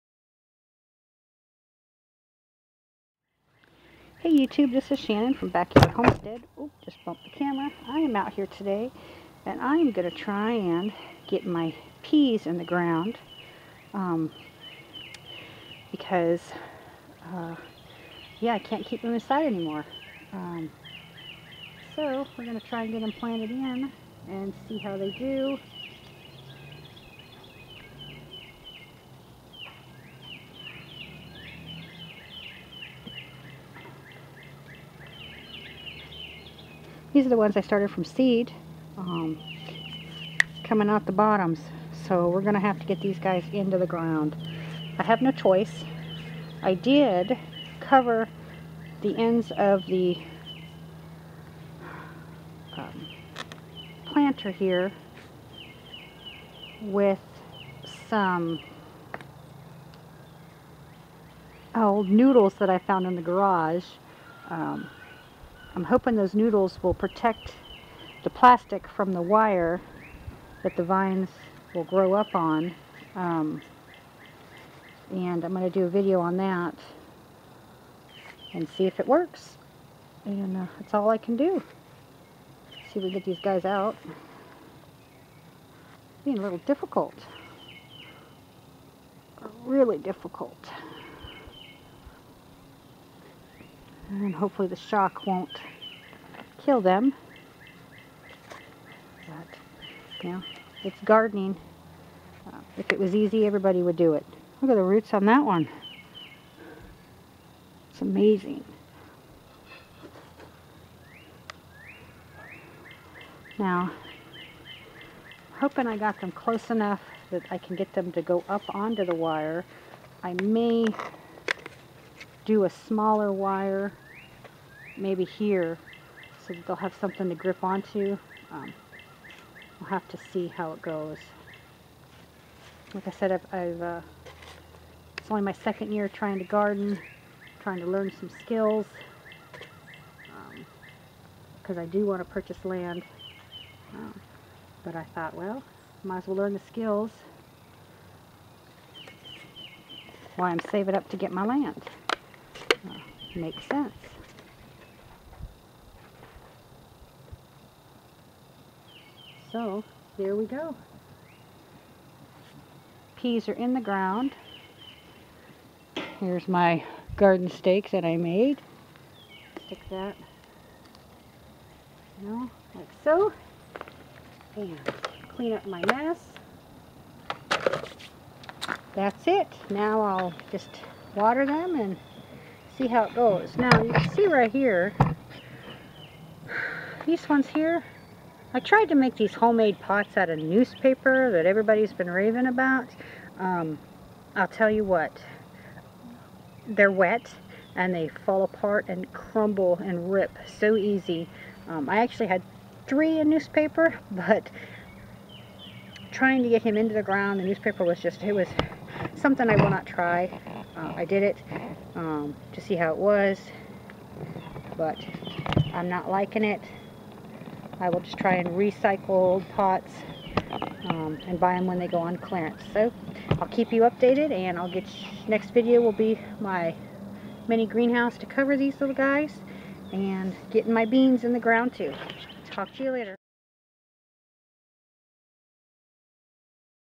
PLANTING AND GROWING PEAS
Planting and growing peas. Today I transplanted the peas I had started from seed inside to outside. They were getting a bit big and it needed to get done. I have planted 12 plants today in this container. The container is a 55 gallon barrel cut long ways. I have added 2x2 on the sides to hold up the wire for the plants to climb on. As you can see I tryed to do the newspaper method for planting, but I really didnt like it. Tommarrow Ill show you how I cover them at night now, as it still might get a little cold here.